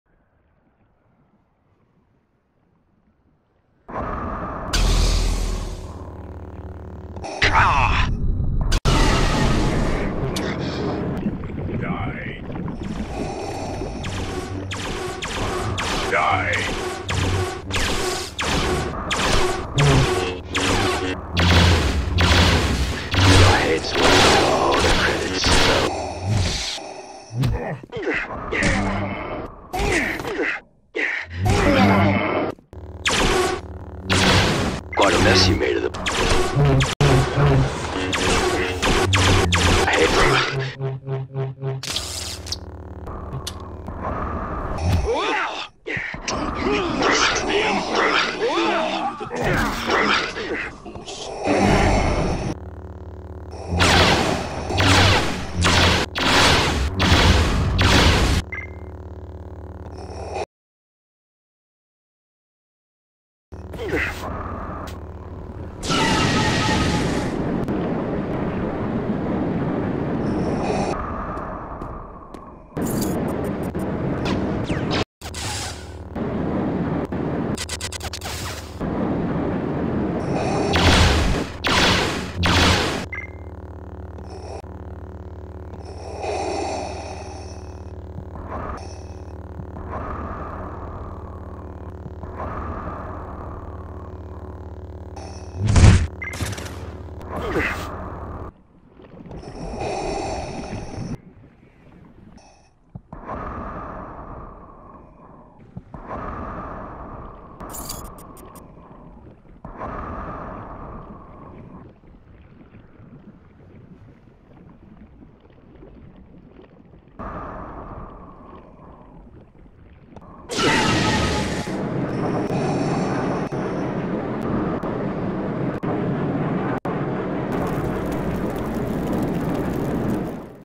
Boba Fett VS Darth Vader (Luke's OLD Unfinished Machinima Project)
This is the earlier version of our planned (and almost finished) machinima short film based on the comic book Star Wars: Enemy Of The Empire where we see the fight between Darth Vader & Boba Fett. As you can see this is completely unedited and only a draft which by the way was made almost 2 years ago. The newer version of it which you will soon get to see has almost all the scenes re-shot and edited, but since Luke also wasted a lot of time on the original here we thought why let it go to waste, so here it is!

This video was created and edited in the VideoPad video editor in Serbia 2023.

Remember guys: God is awesome all the time , may the force be with you always and we will see you in another video you REBEL SCUM!!! ;)